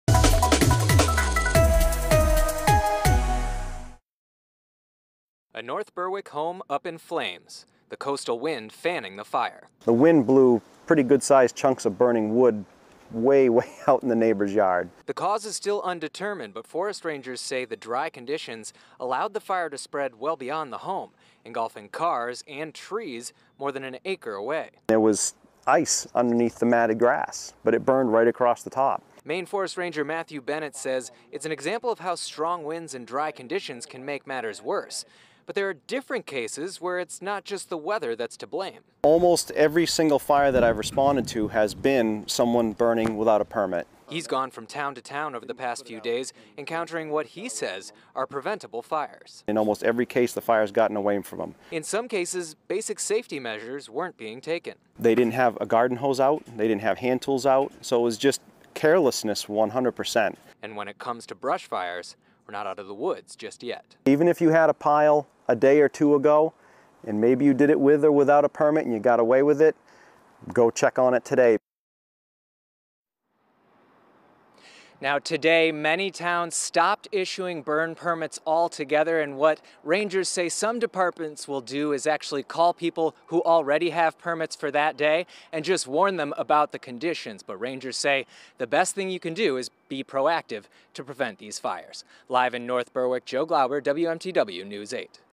Much of southern Maine at elevated fire danger risk
It was a busy weekend for crews that battled brush fires across southern Maine. Much of the area remained under a high risk on Monday.